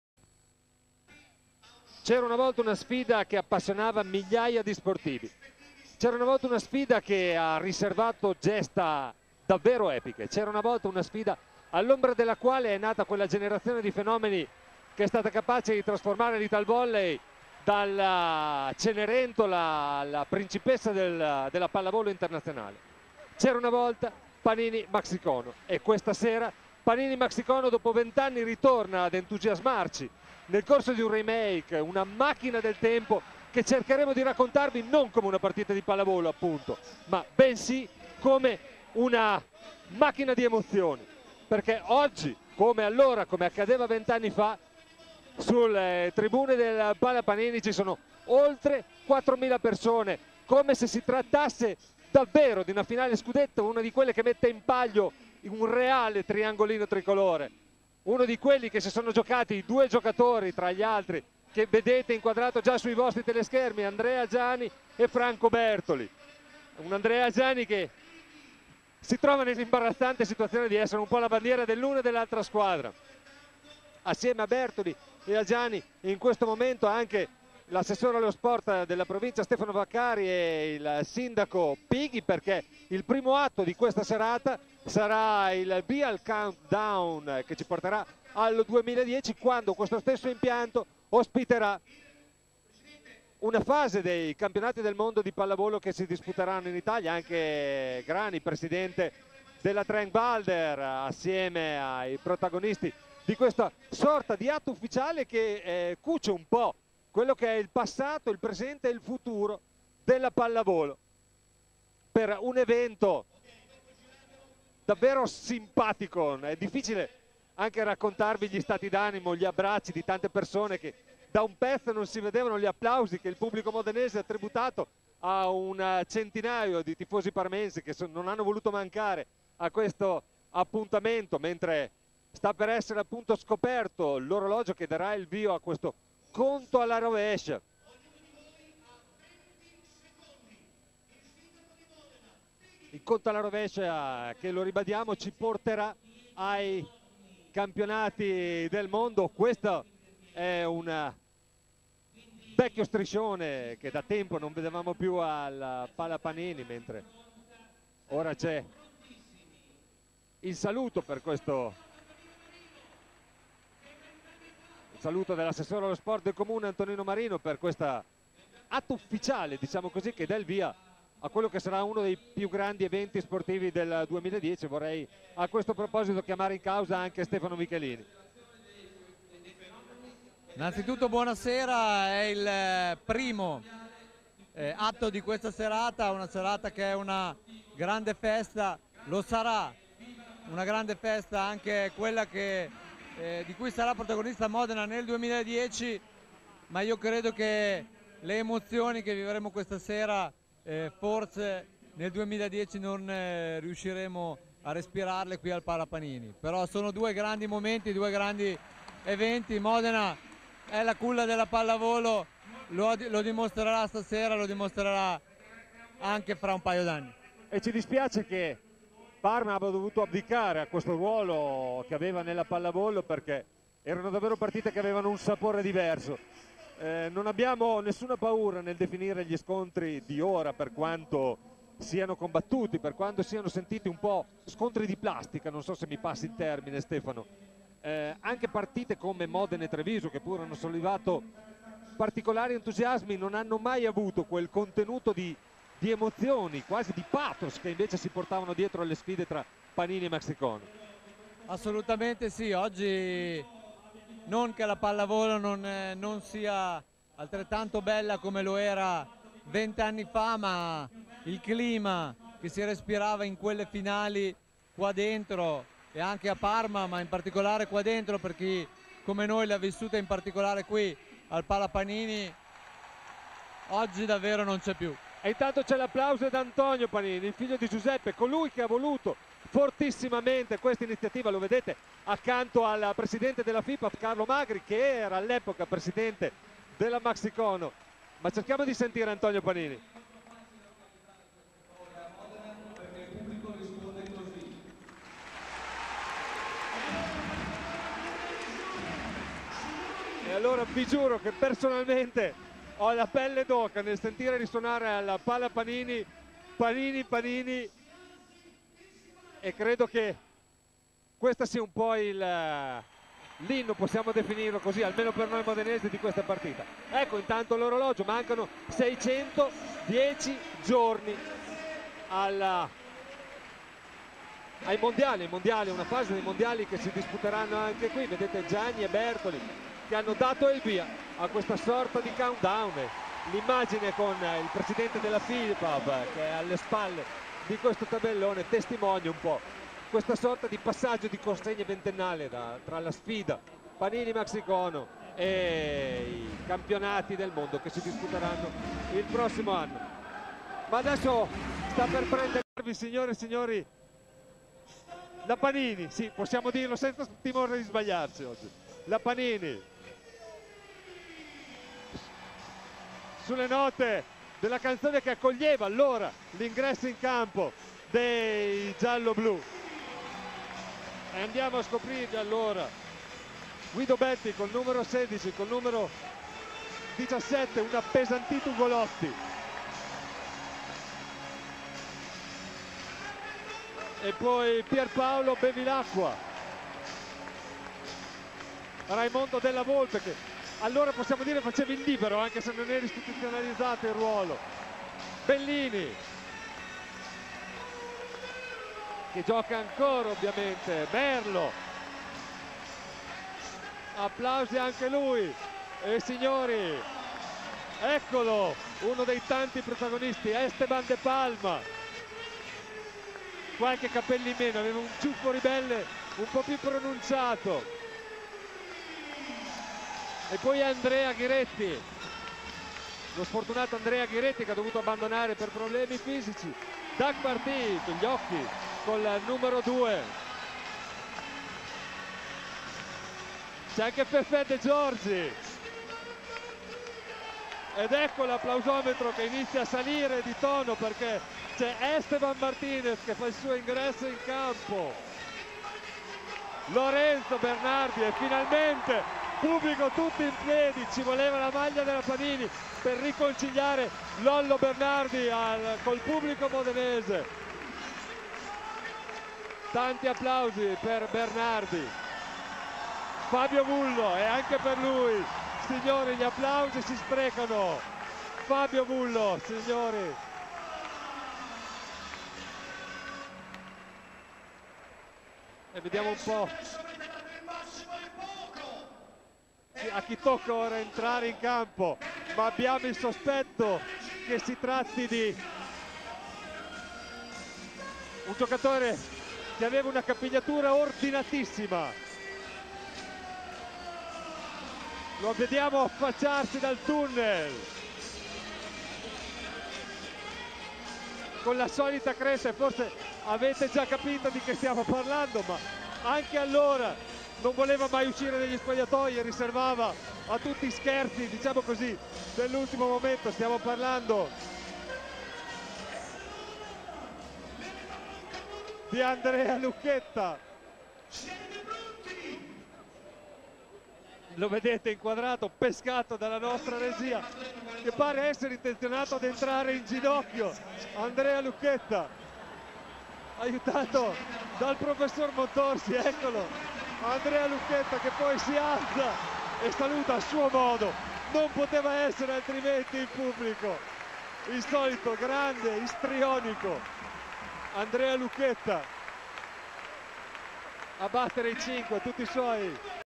Modena Panini Maxicono 220109 1
La sfida delle stelle Modena Panini Maxicono 220109 accensione dell'orologio per il countdown dei mondiali